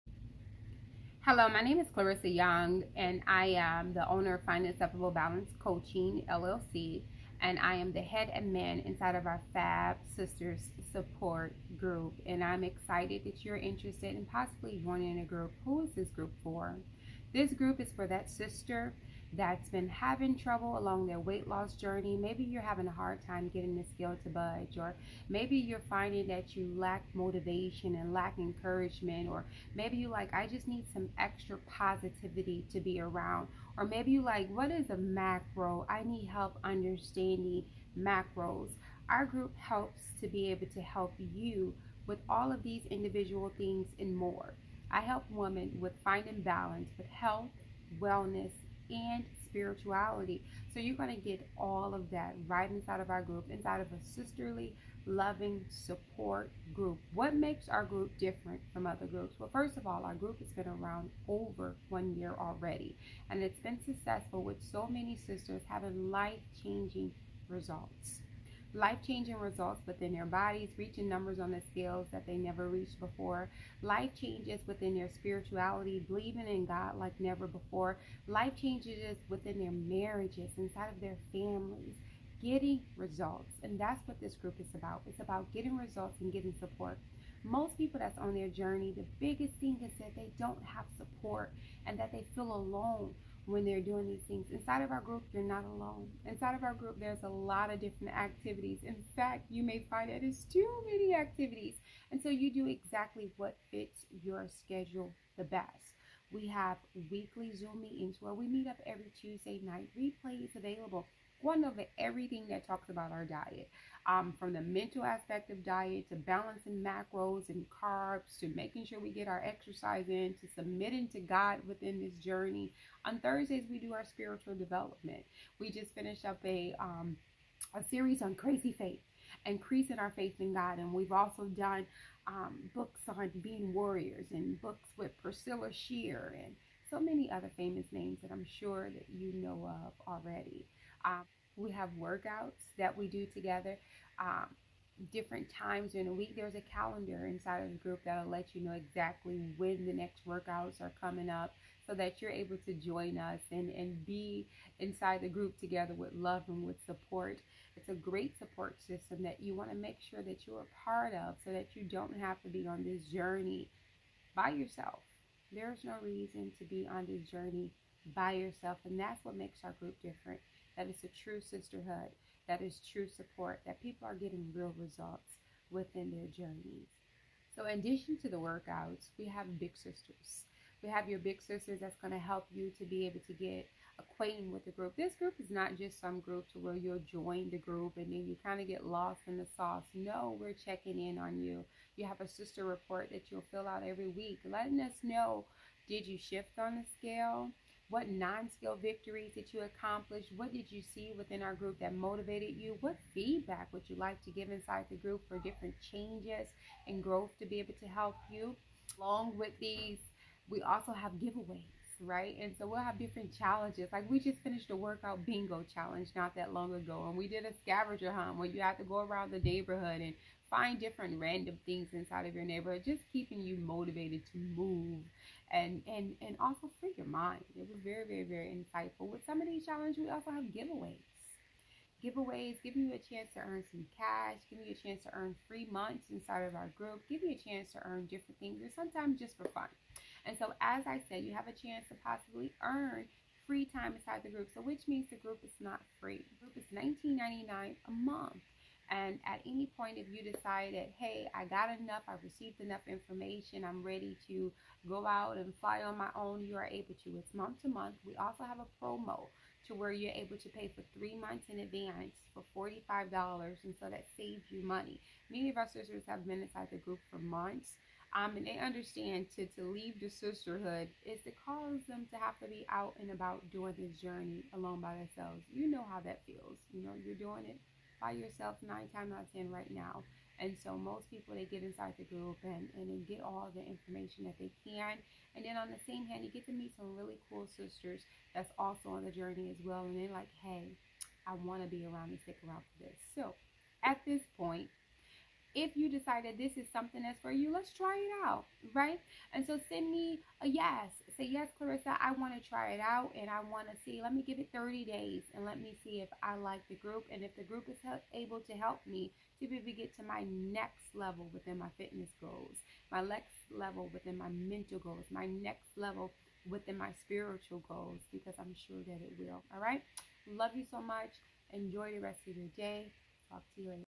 Why should you join FAB SISTERS SUPPORT GROUP?
Benefits of FAB SISTERS SUPPORT GROUP:
•Weekly FAMILY Zooms- Education, encouragement, guidance and more.
•Weekly Spiritual Zooms- tools to grow spiritually. Using books and/or online material.
•Accountability- Weekly Weigh-Ins used to motivate you to stay focused. Providing praise when you do well,  encouragement when needed, and reaching out when you are missing.
•Support- Environment with other like-minded women going after their goals.
•Big Sister- Guides you through group, answer questions, and give you a more intimate support. She relates to me your needs and concerns for me to address appropriately
•and more!
In addition you have access to all services outside of group, such as FREE workouts on zoom.  (replays on YouTube). This group can easily be valued at over $99 a month. However it is only $19.99/month.

Would you like to join the community?